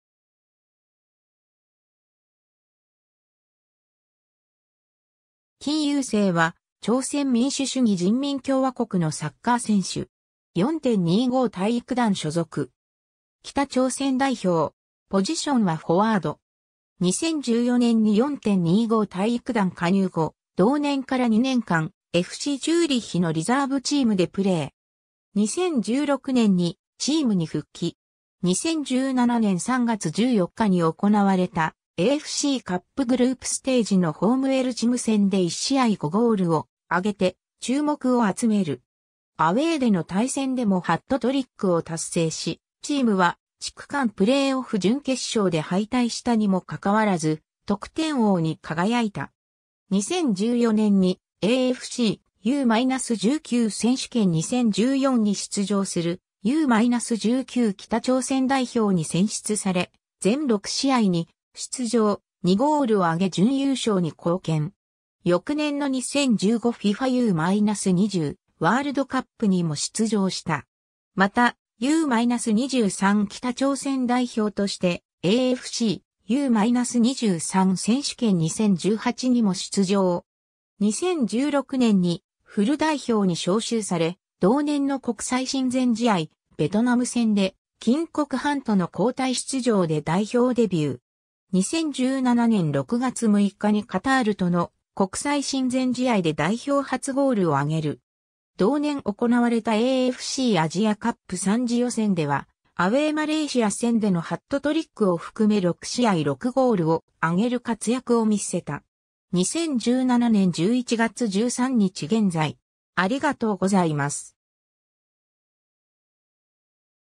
金有成
金有成（キム・ユソン、朝鮮語: 김유성、英語: Kim Yu-Song、1995年1月24日 - ）は、朝鮮民主主義人民共和国（北朝鮮）のサッカー選手。
4.25体育団所属。
北朝鮮代表。
 2014年に4.25体育団加入後、同年から2年間、FCチューリッヒのリザーブチーム (FC Zürich II、スイス・プロモーションリーグ所属) でプレー。
2016年にチームに復帰。
 2017年3月14日に行われたAFCカップグループステージのホーム・エルチム戦（綾羅島5.1スタジアム）で1試合5ゴールを挙げて注目を集める。
アウェイでの対戦でもハットトリックを達成し、チームは地区間プレーオフ準決勝（全体の準々決勝）で敗退したにもかかわらず得点王に輝いた。
 2014年にAFC U-19選手権2014に出場するU-19北朝鮮代表に選出され全6試合に出場、2ゴールを挙げ準優勝に貢献。
翌年の2015 FIFA U-20ワールドカップにも出場した。
また、U-23北朝鮮代表としてAFC U-23選手権2018にも出場。
 2016年にフル代表に招集され、同年の国際親善試合・ベトナム戦で金国范との交代出場で代表デビュー 。
2017年6月6日にカタールとの国際親善試合で代表初ゴールを挙げる。
同年行われたAFCアジアカップ3次予選ではアウェイ・マレーシア戦でのハットトリックを含め6試合6ゴールを挙げる活躍を見せた。
 2017年11月13日現在